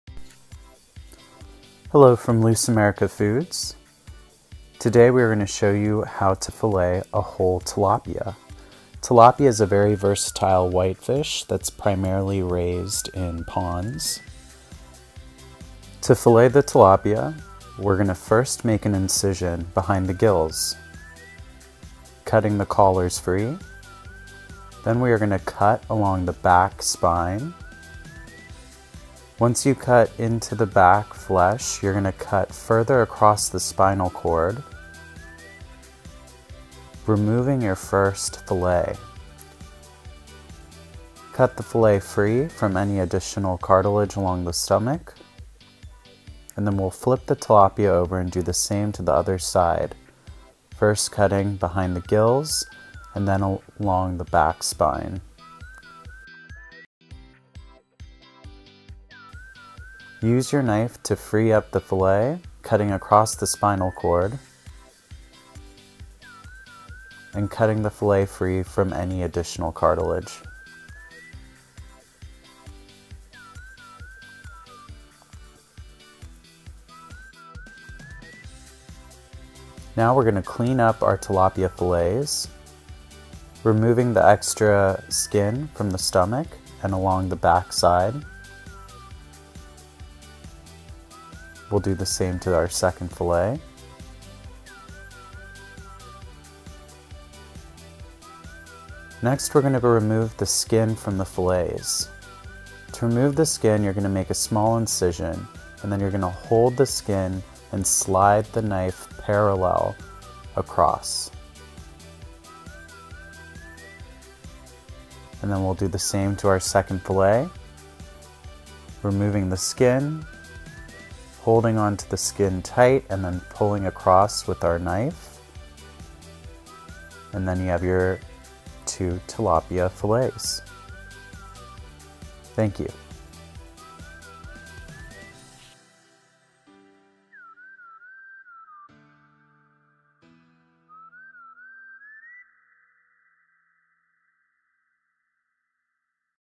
How to Fillet Whole Tilapia
While there are many ways to fillet whole fish, Dolores from our team has been cutting fish for 30+ years and shows us his method for filleting whole tilapia. Visit Lusamerica.com to learn more.